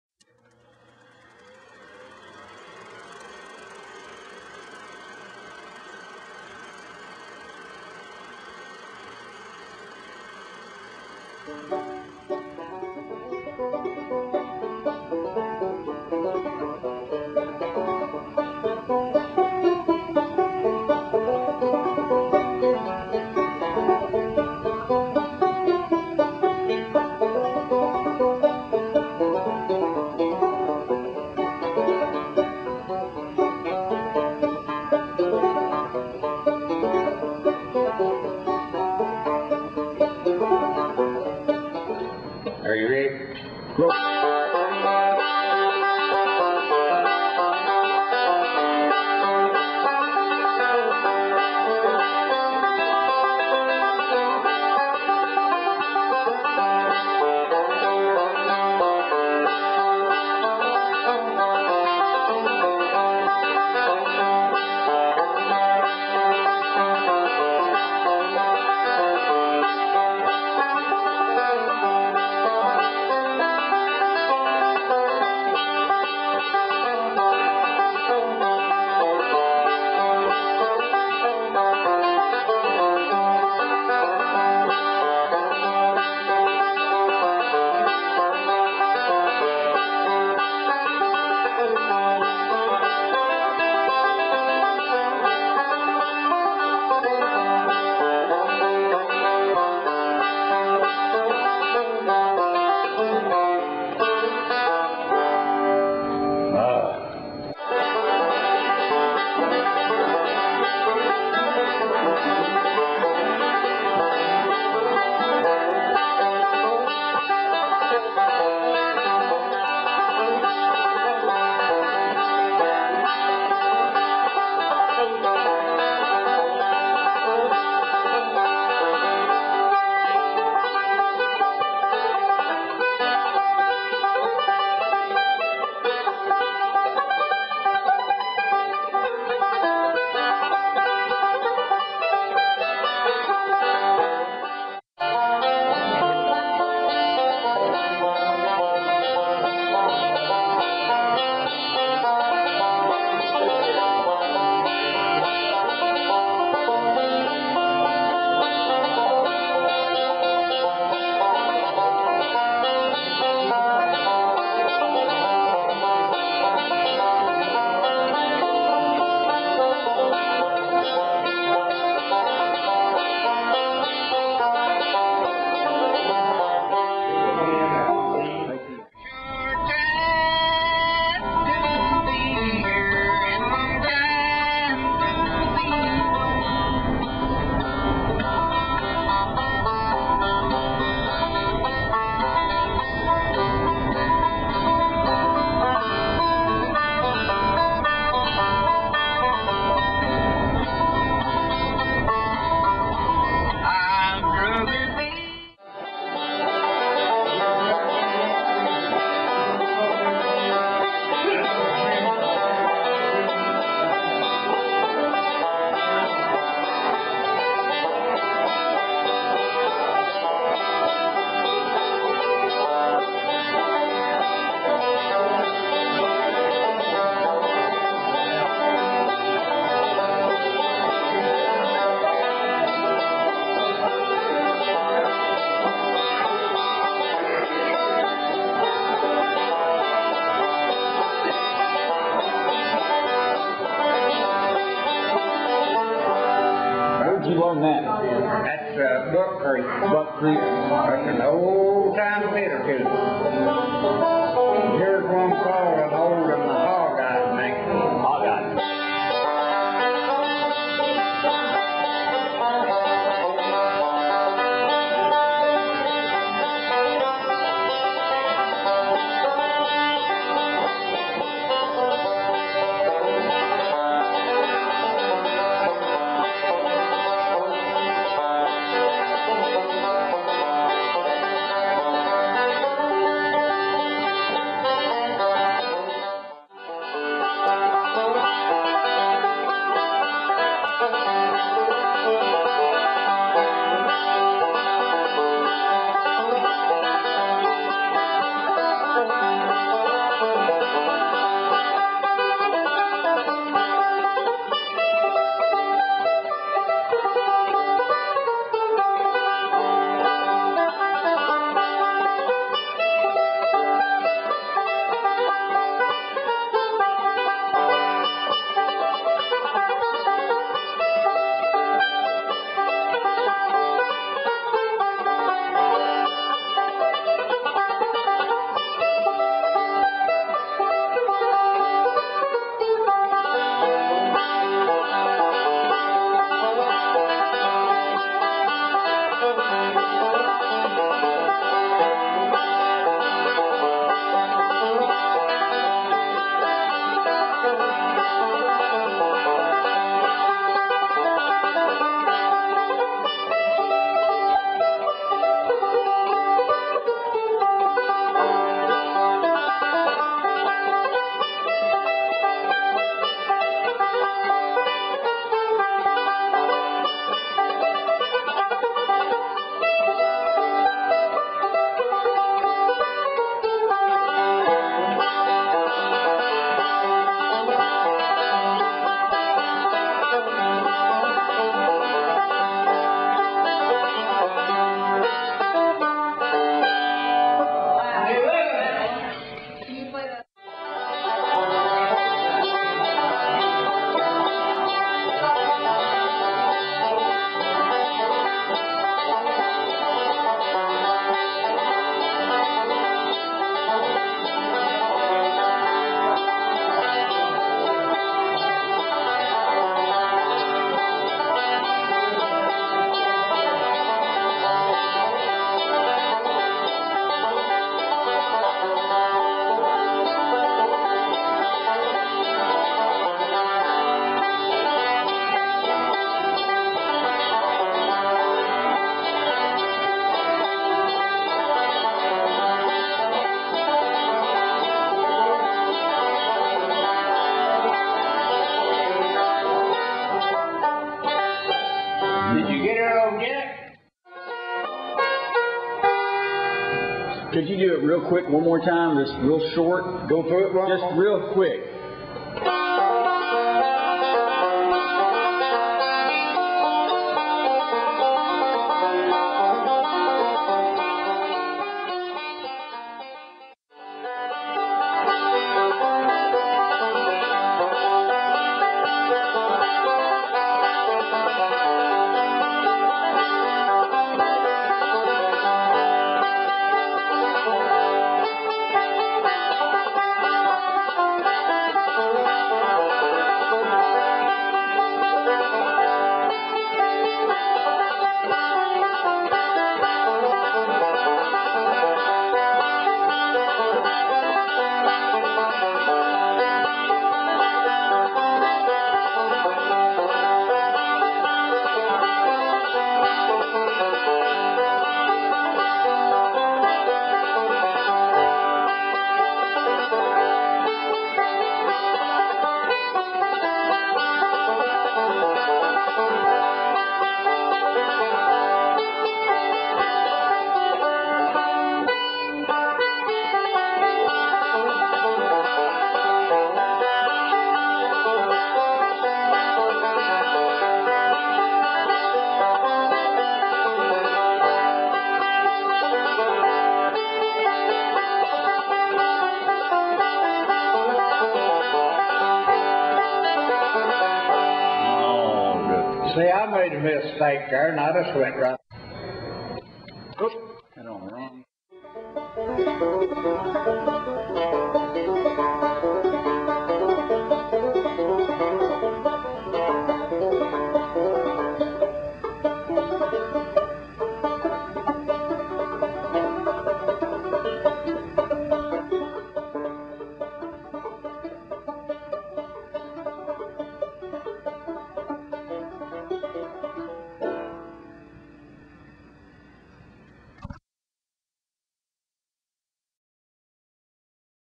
Lee "Boy" Sexton and his Banjo (lo-rez)
I traveled to Line Fork KY to film the great Lee Sexton and his Kentucky "Two-Finger" style of playing the 5-string banjo.  This is a demonstrational video of his unique blend of old-time and bluegrass banjo picking.  Enjoy!